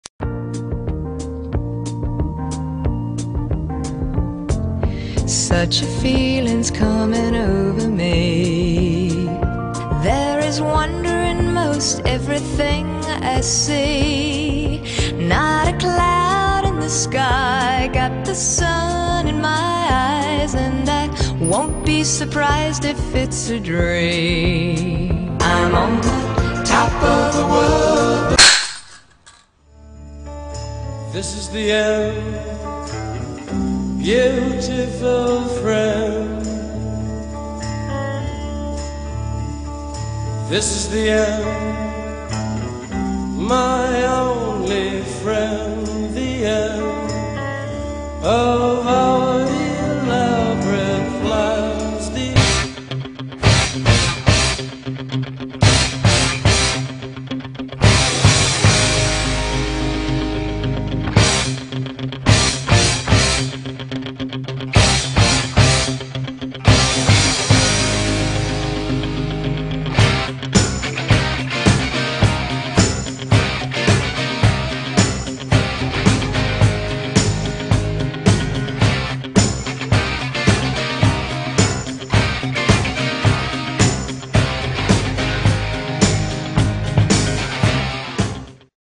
Nolan`s Cheddar Cheese Commircial
a funny commercial from a cheese company XD